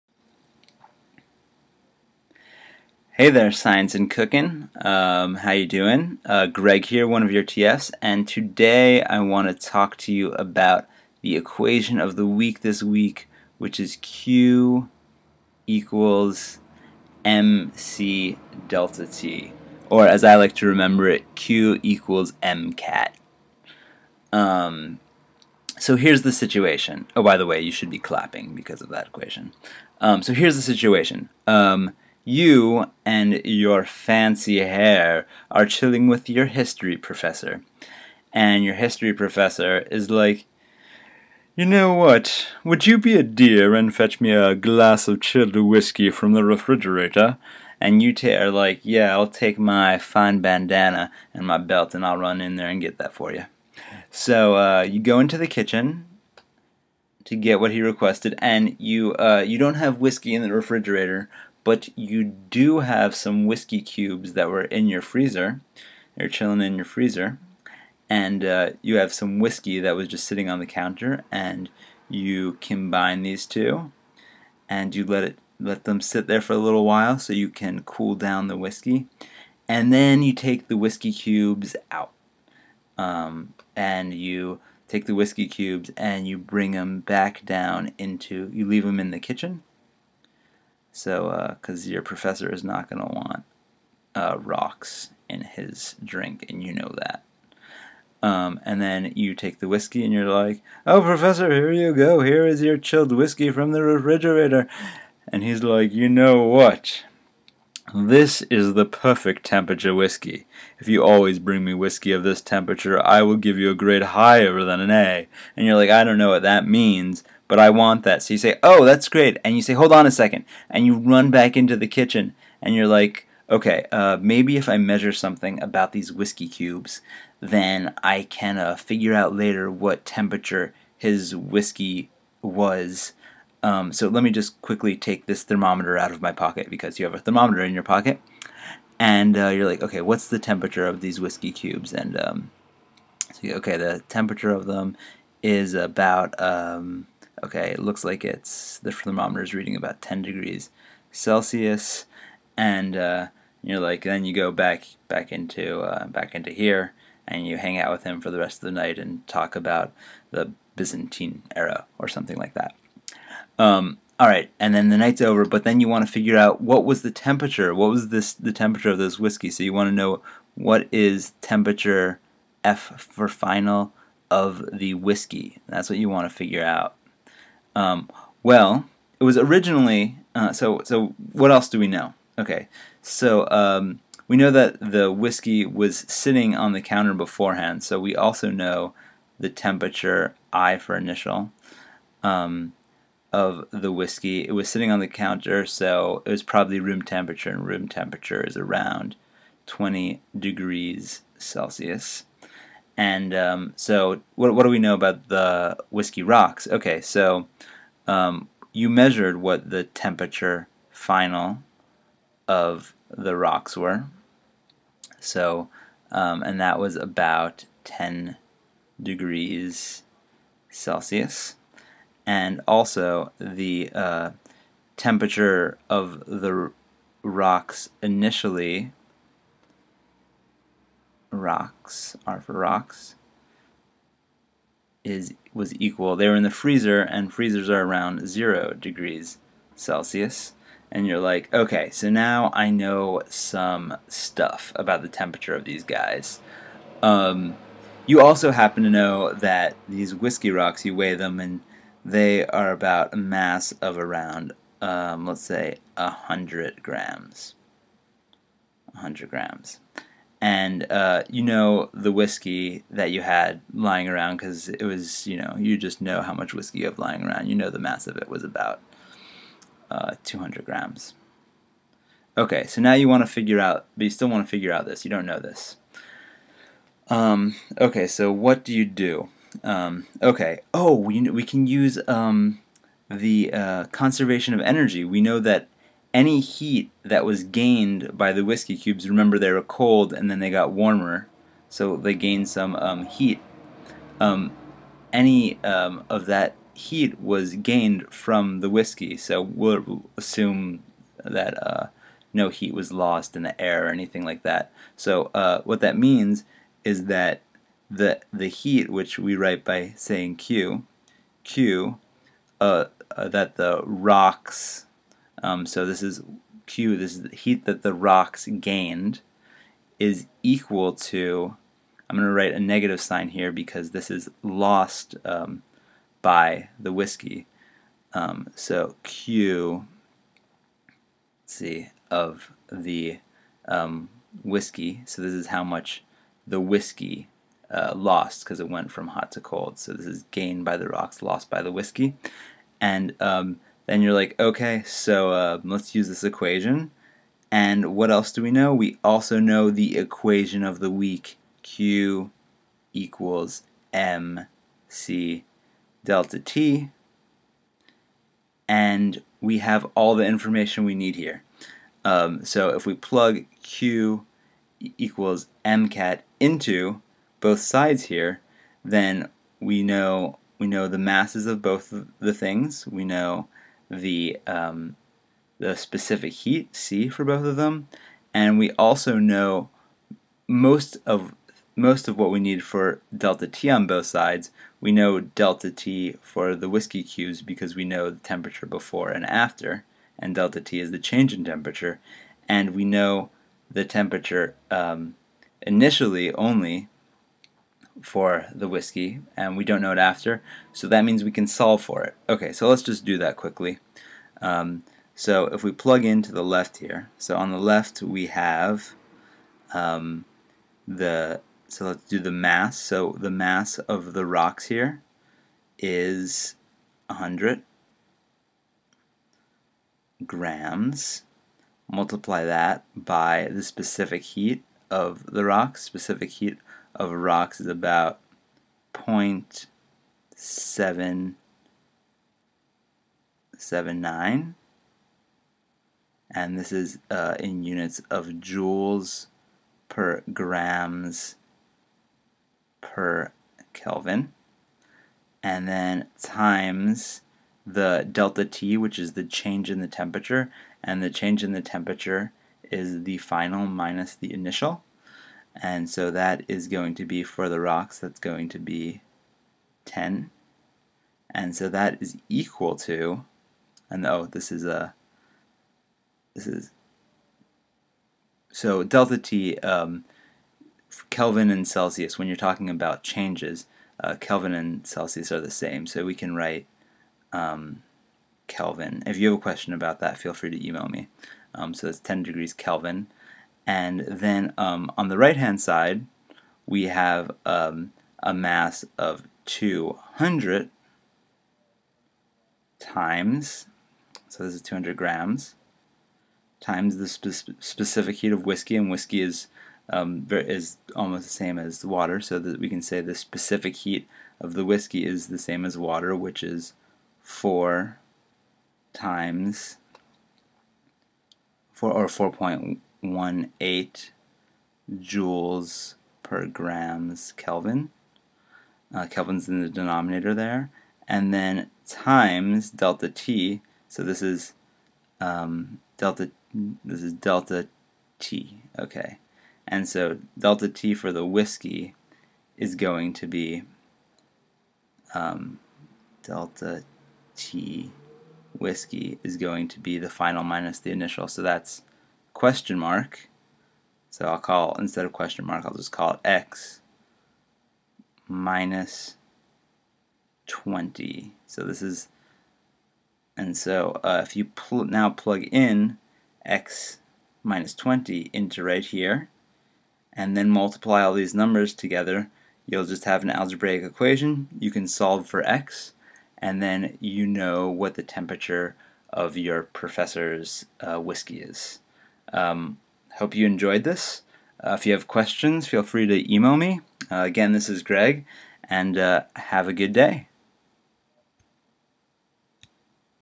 Q = m c ΔT
What's the connection between heat and energy, and why does it matter for cooking?